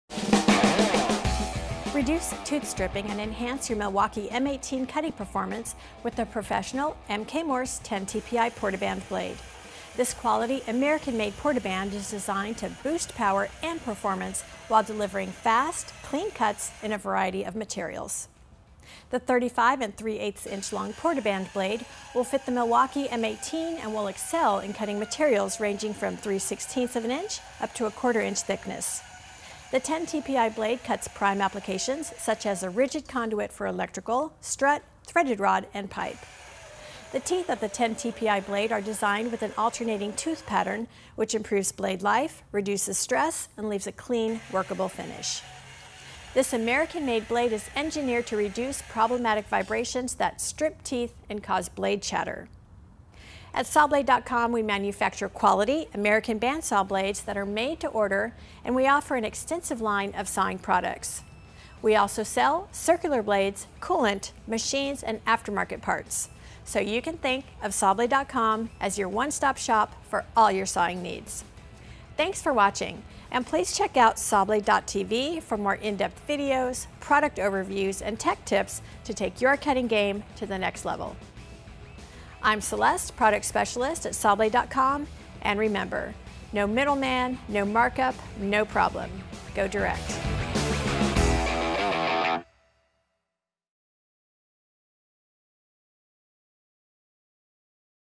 M.K. Morse ZWEP3510R 35-3/8 in. x 10 tpi Bi-Metal Portable Band Saw Blade
You deserve the best portaband blade on the market.

This american-made blade will deliver results, not excuses!

Specifications:

Brand: MK Morse
Mfr. Part Number: ZWEP3510R
Blade Length: 35-3/8 in.
Teeth Per Inch: 10
Blade Width (Inch): 1/2
Blade Thickness (Decimal Inch): 0.0200
Blade Material: Bi-Metal
Cutting Edge Style: Toothed Edge
Tooth Material: High Speed Steel
Back Material: Alloy Steel
Tooth Set: Raker
Pitch Pattern: Straight

Ideal for: rigid conduit for electrical strut, threaded rod, and pipe.

Works best in materials ranging from 3/16” up to 1/4” thick.

Additional Features:

Extended Blade Longevity
Reduced Blade Stress and Vibration
Leaves a Clean Workable Finish
Lessened Tooth Stripping
Lessened Blade Chatter
Alternate Top Bevel

No Middleman. No Markup. No Problem. Go Direct.